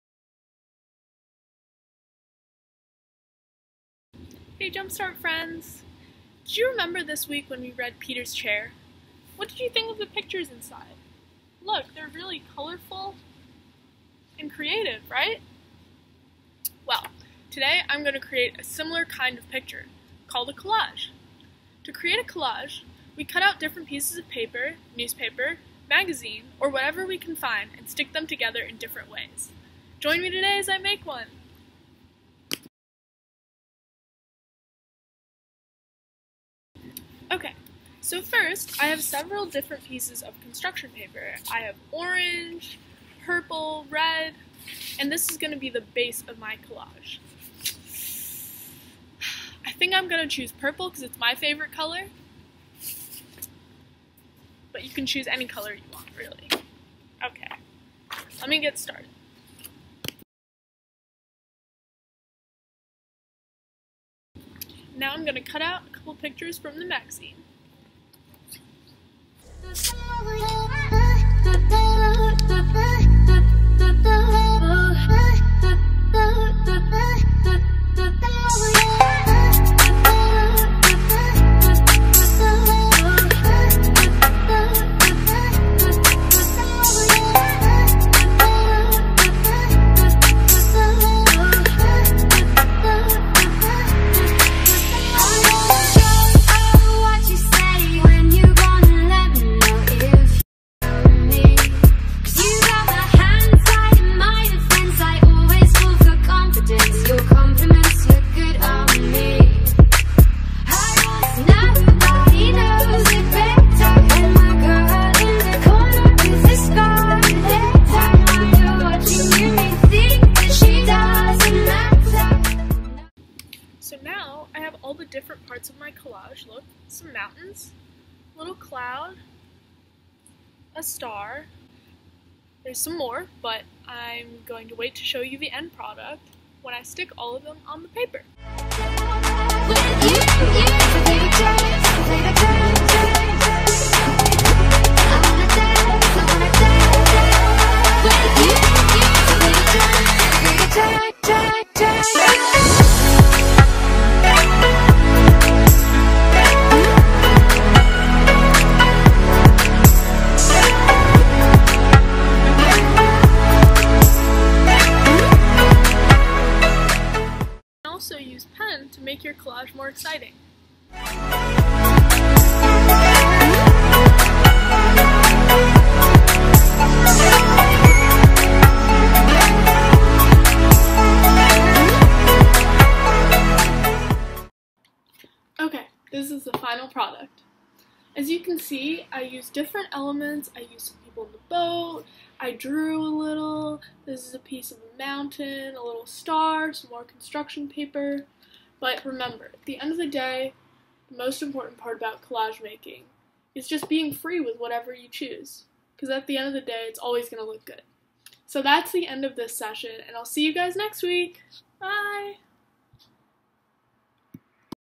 Learn Along: Art Center - Making a Collage
In this video, Miss Jade shows how she creates a collage and gives tips on how Jumpstarters can make their own collages!